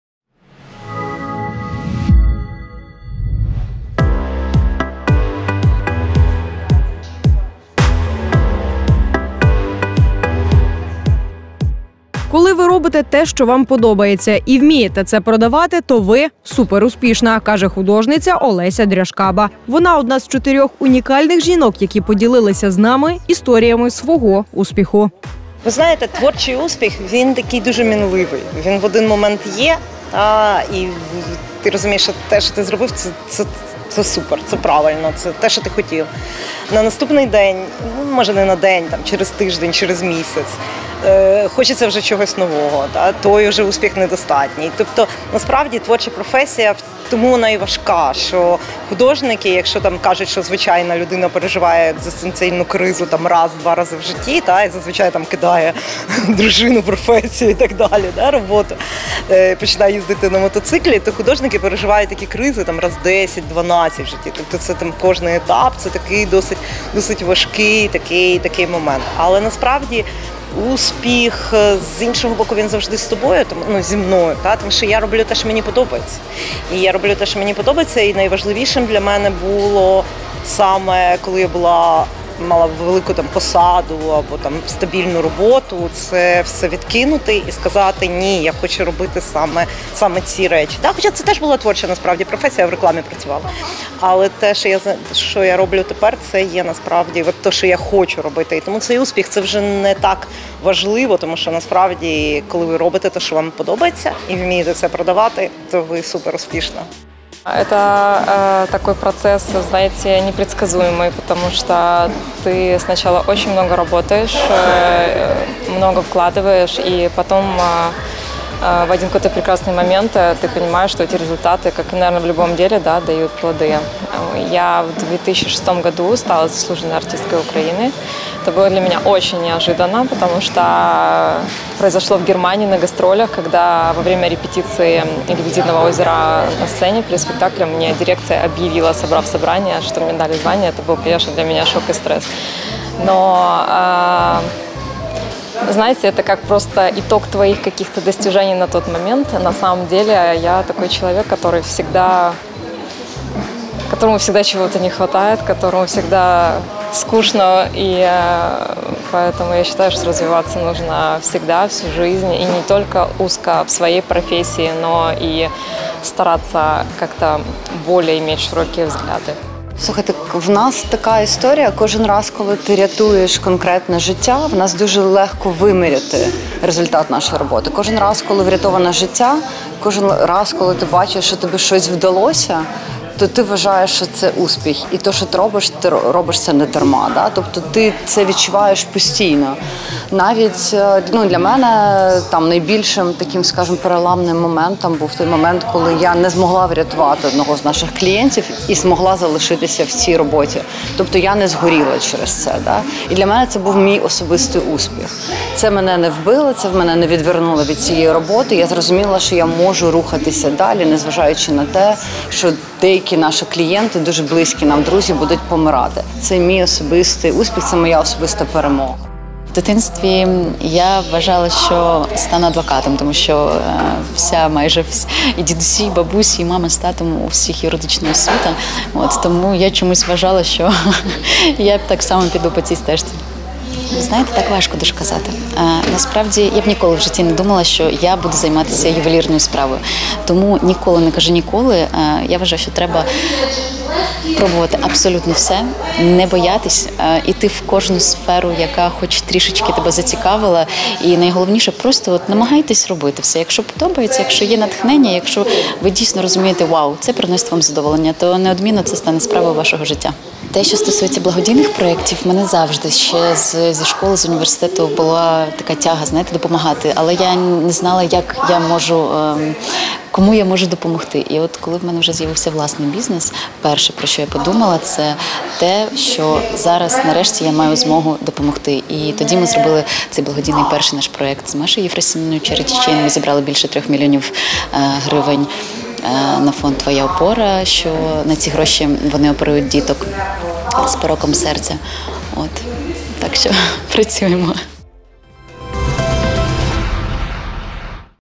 Успішні українки: різний шлях, але однакова любов до справи
«Коли ви робите те, що вам подобається, і вмієте це продавати, то ви суперуспішна», — каже художниця Олеся Драшкаба. Вона одна з чотирьох унікальних жінок, які поділились з нами історіями свого успіху.